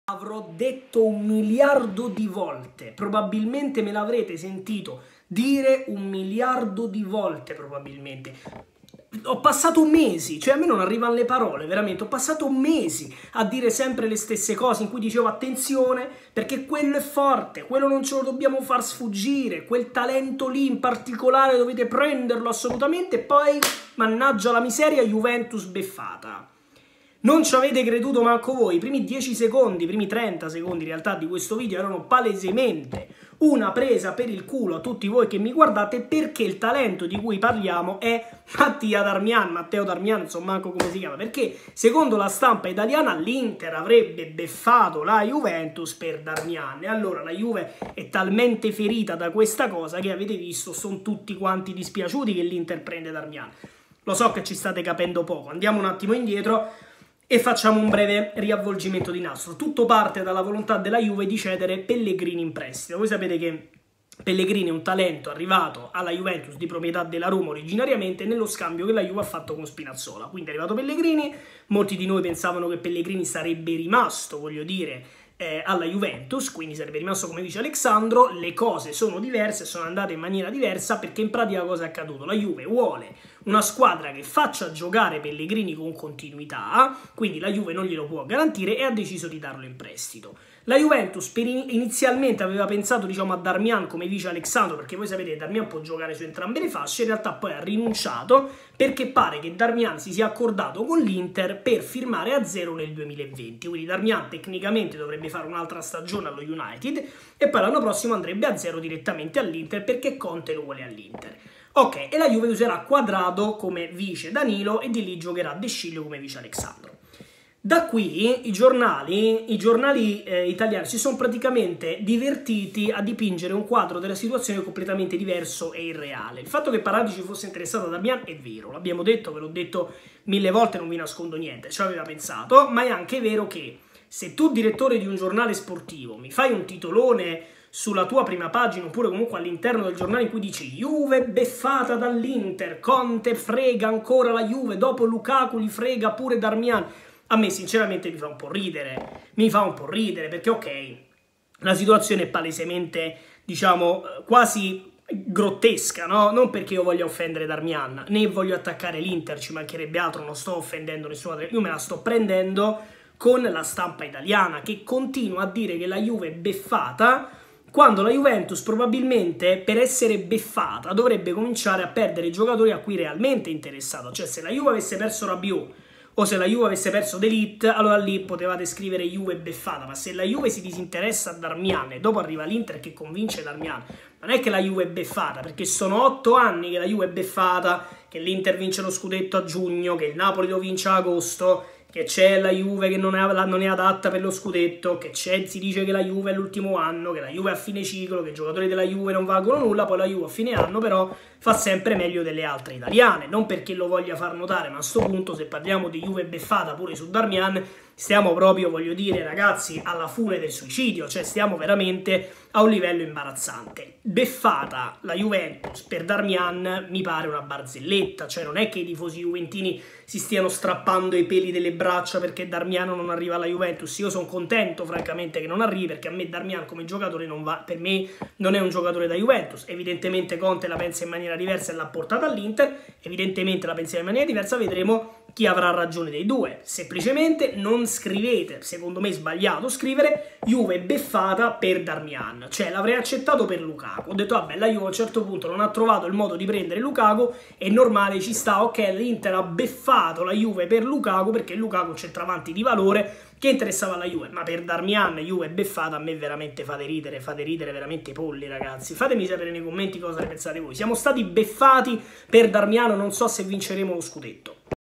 JUVENTUS BEFFATA! JUVENTUS BEFFATA! E ADESSO COME FAREMO?
Per la stampa italiana la Juventus sarebbe stata beffata dall'Inter nell' affare Darmian (😂), un qualcosa di inventato di sana pianta solo per sminuire la Juventus e pompare le concorrenti: disinformazione pura o semplicemente frustrazione accumulata?